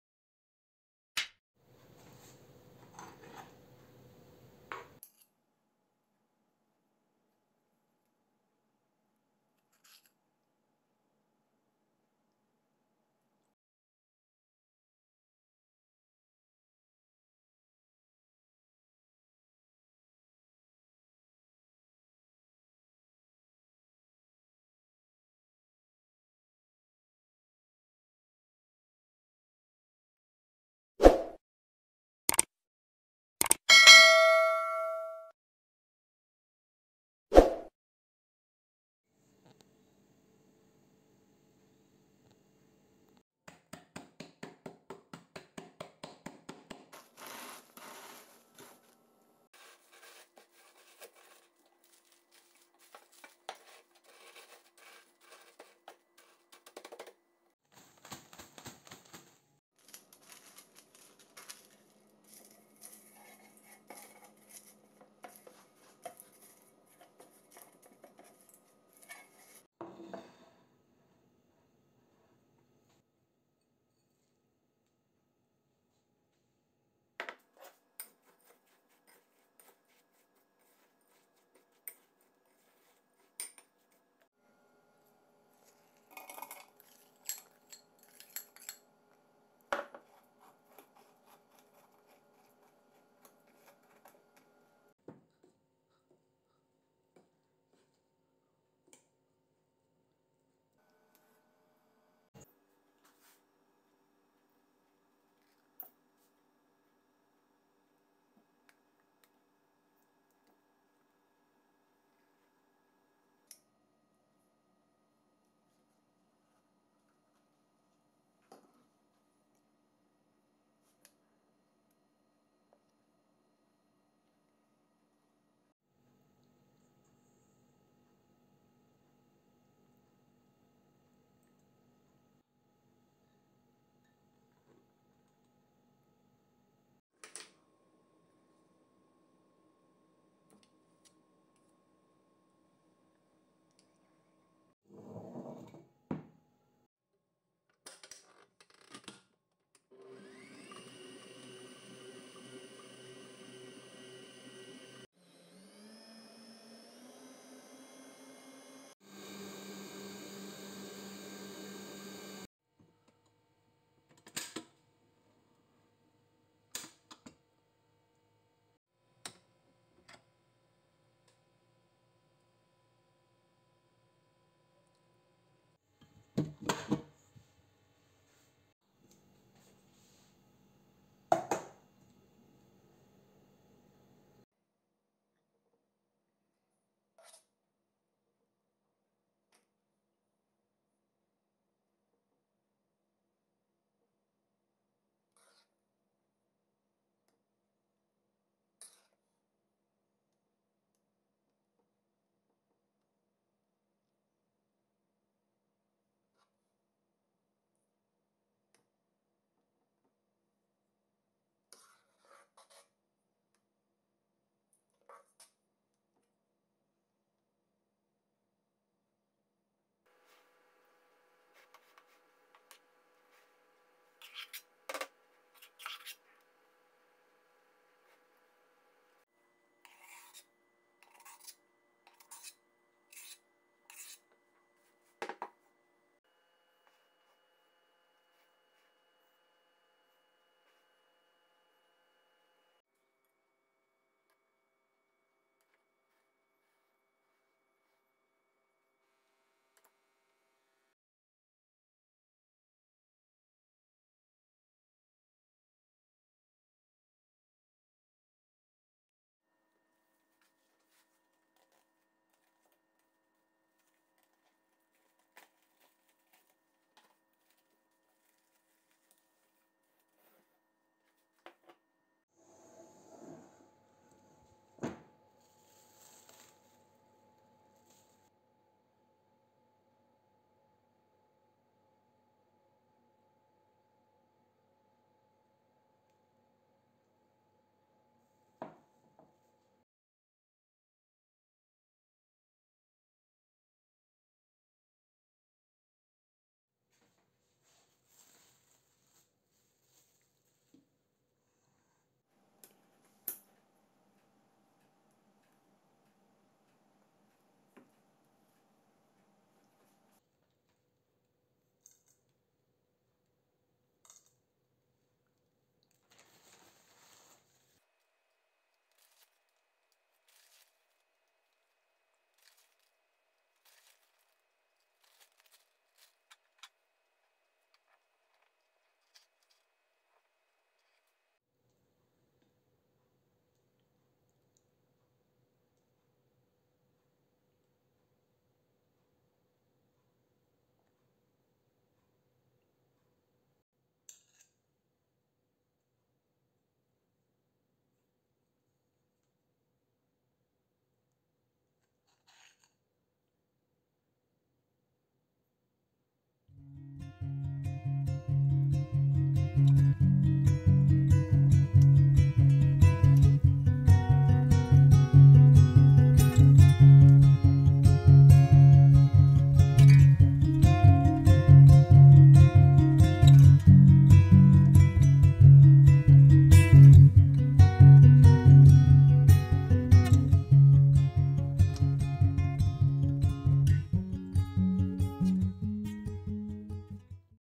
Gâteau mascarpone à la pâte de spéculoos sans cuisson.
250 g de spéculoos
75 g de beurre
40 cl de crème fleurette 30%
Pâte spéculoos
4 c à s de sucre glace

Je vous remercie infiniment pour votre soutien.